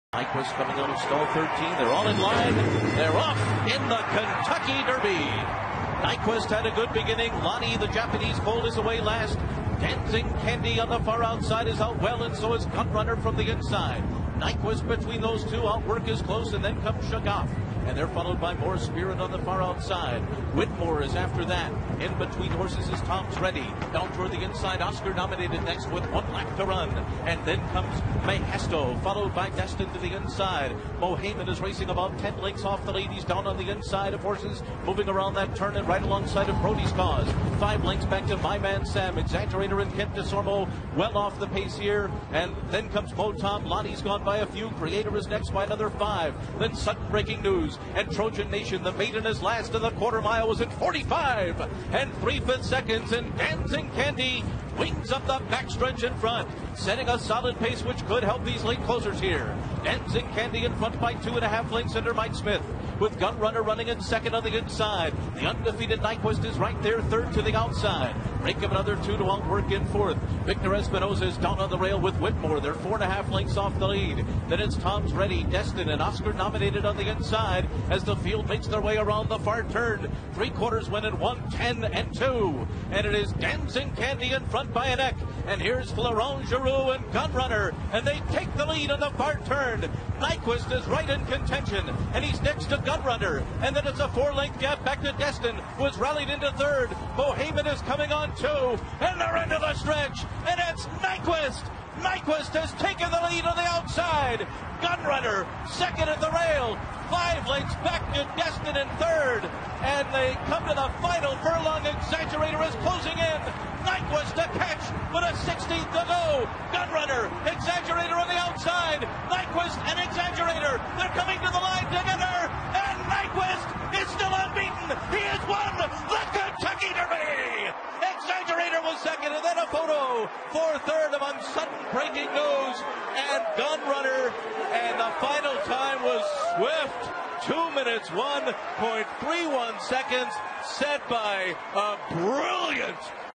Дерби Кентукки 2016 Nyquist
Nyquist одержал победу Кентукки Дерби 2016
Резвость скачки 2.01,31